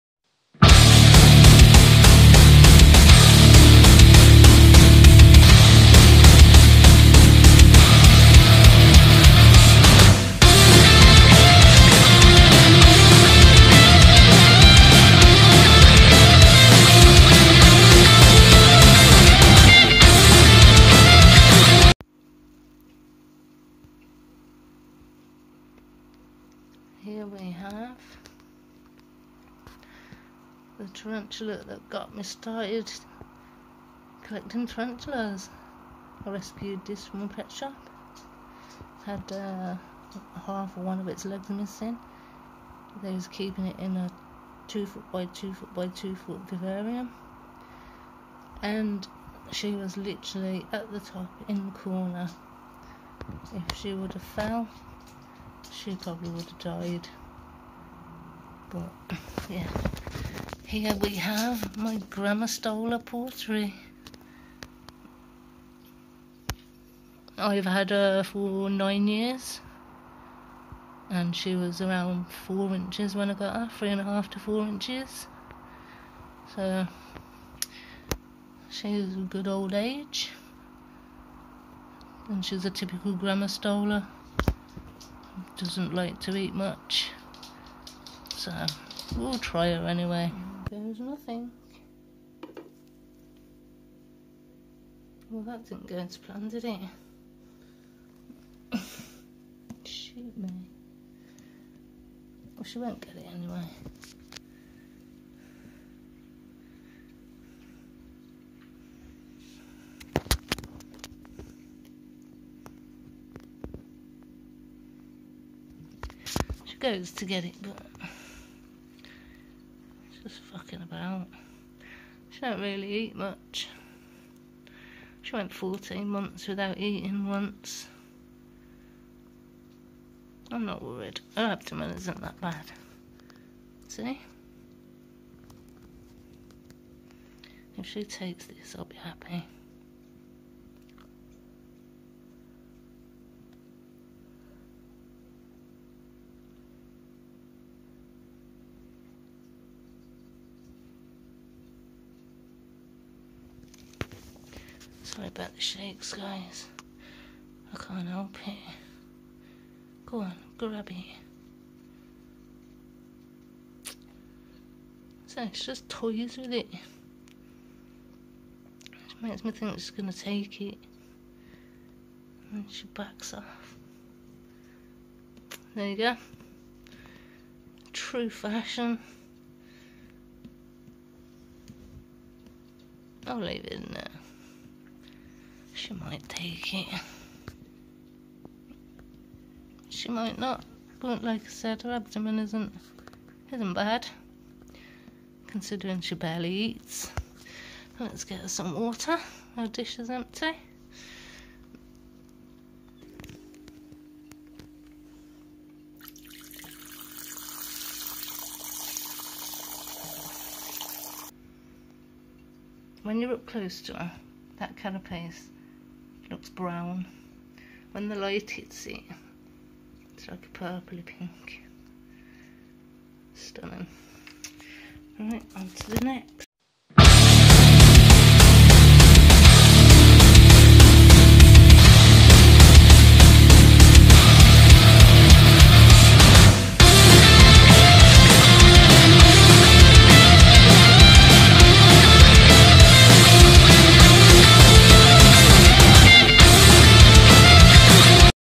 Grammostola porteri my oldest tarantula
Here we have the girl that got me hooked on keeping tarantulas my Grammostola porteri she is roughly 16 + year's old ive had her for 9 yrs she was close to 4 inches when i purchased her i hope you enjoy watching her as much as i do.